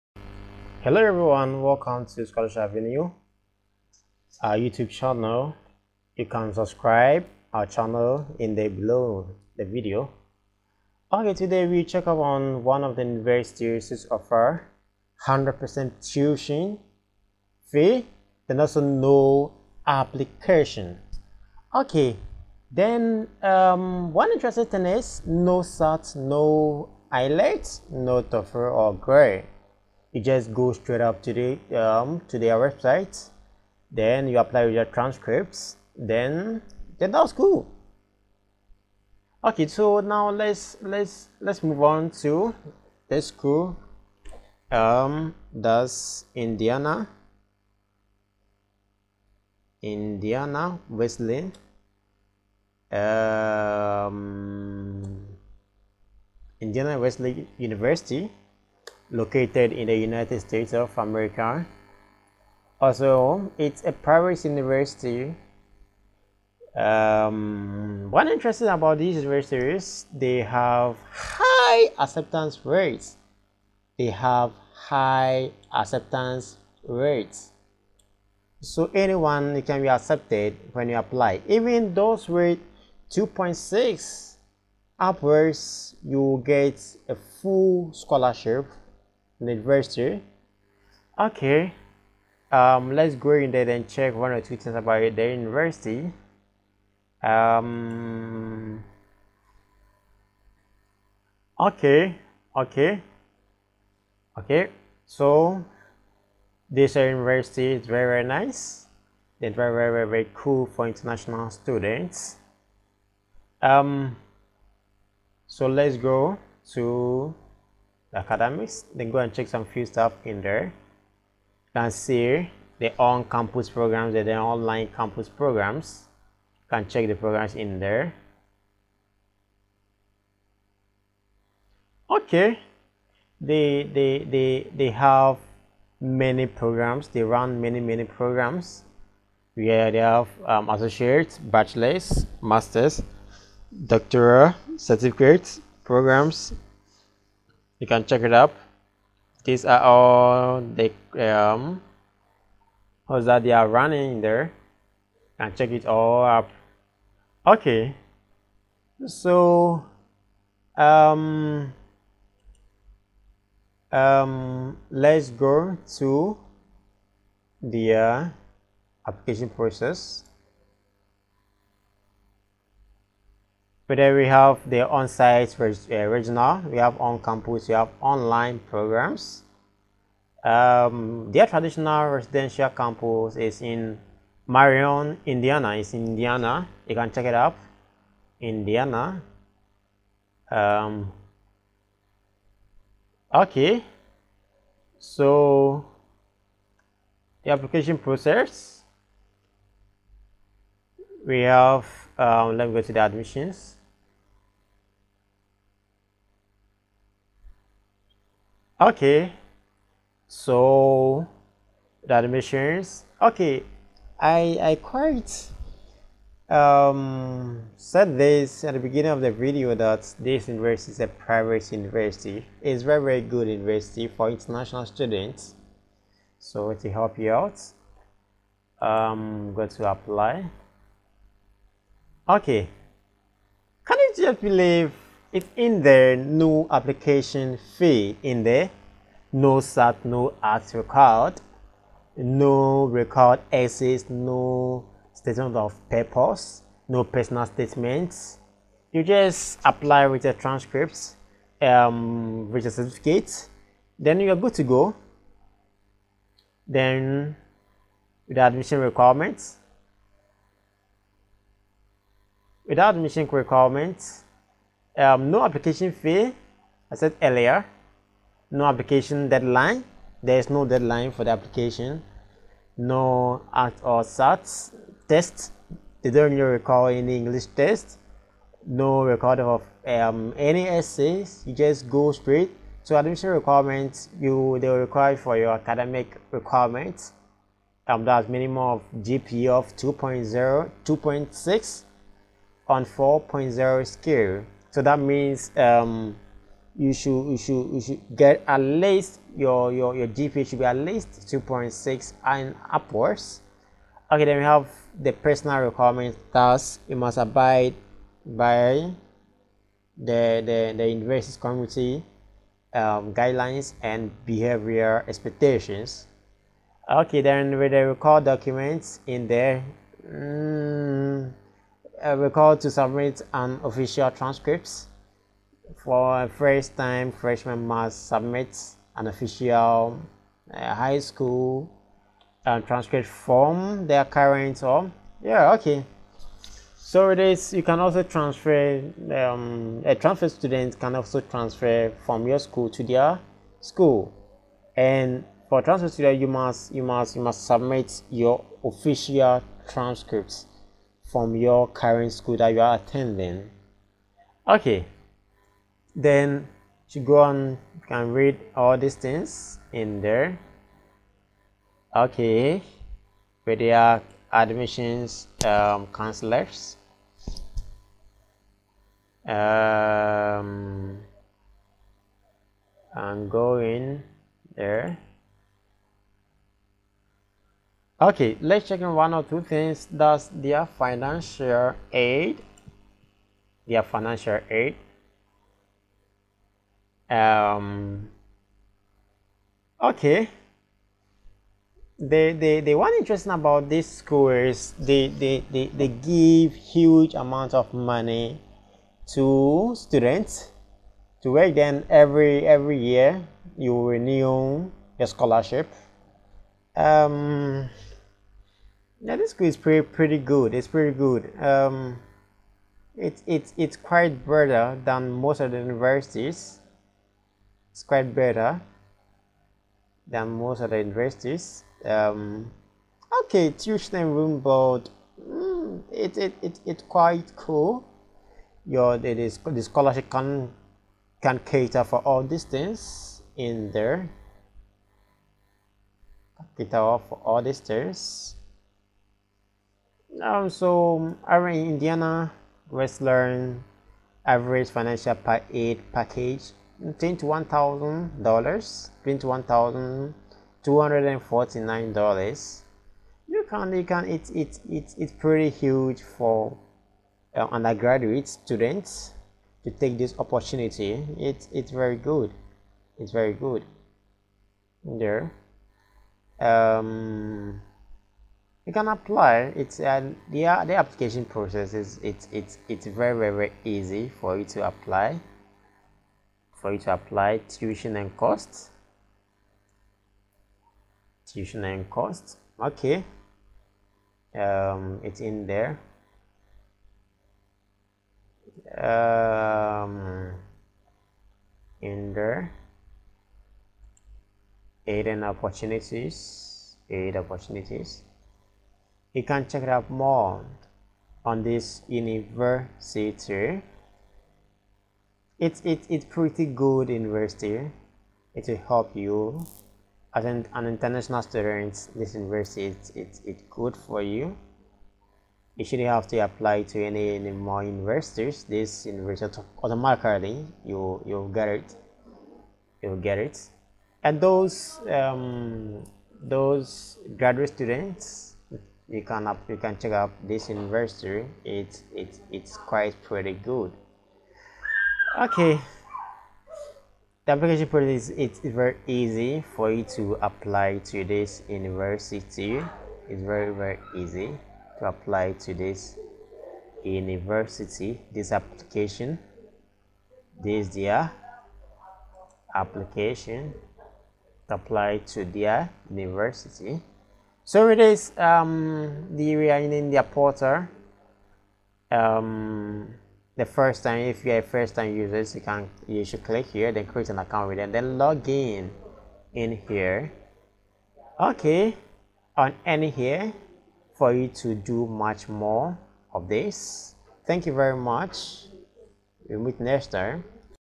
No Application fee, Automatic Scholarship for international students to study in USA
Indiana Wesleyan University offers No Application fee and Automatic Scholarship for international students to study in USA